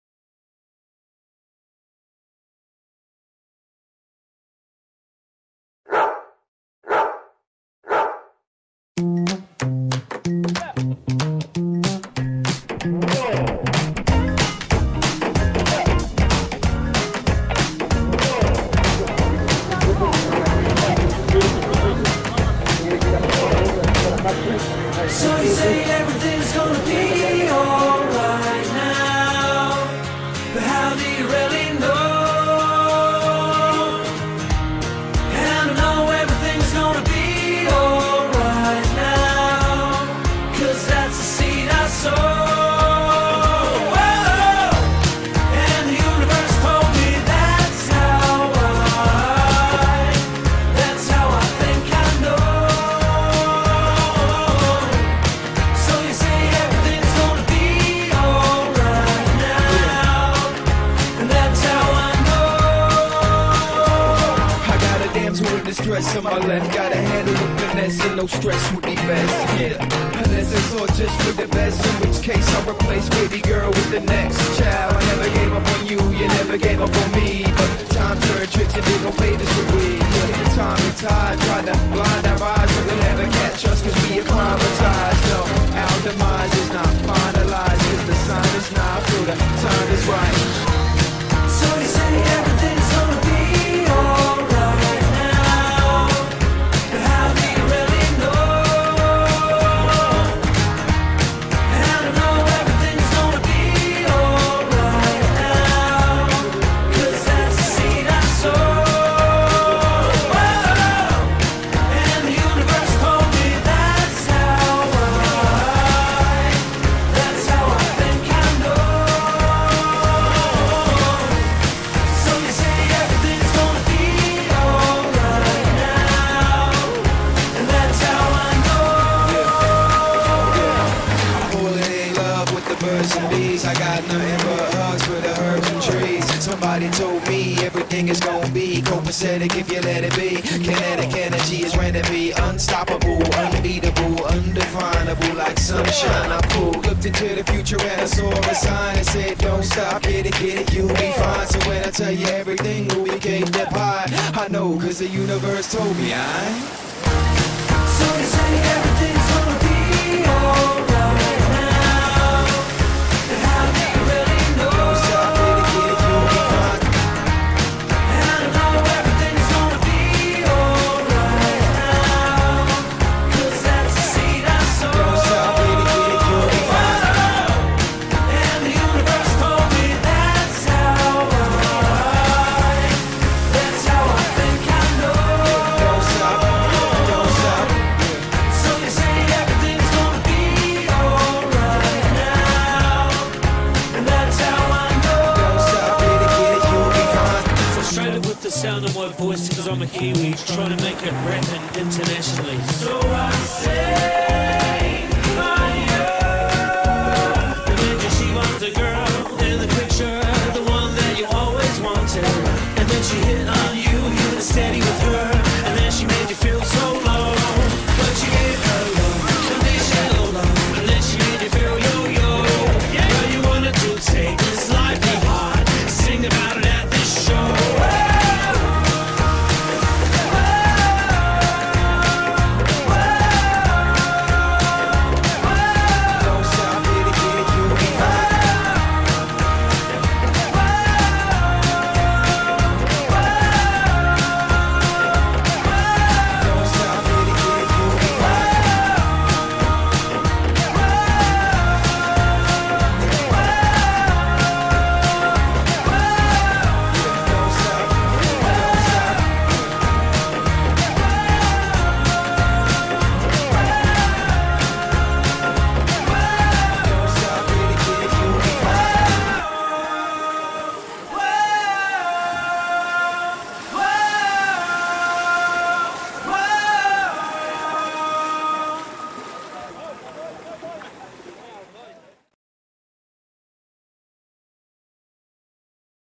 Takapuna Beach Cup Relay part 1.
Takapuna Beach Cup Single and W2 relay race 2012